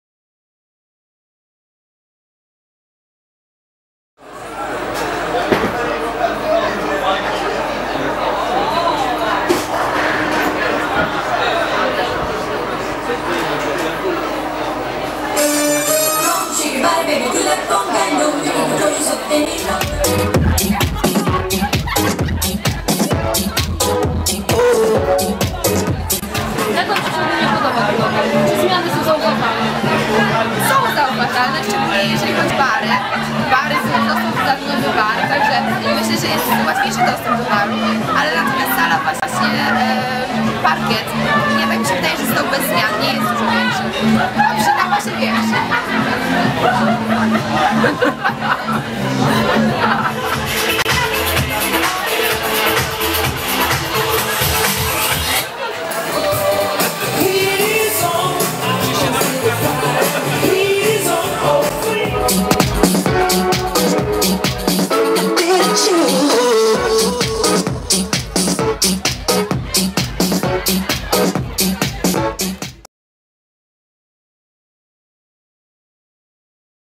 Otwarcie klubu Grand w Rzeszowie
Po długim remoncie otwarto klub Grand w Rzeszowie.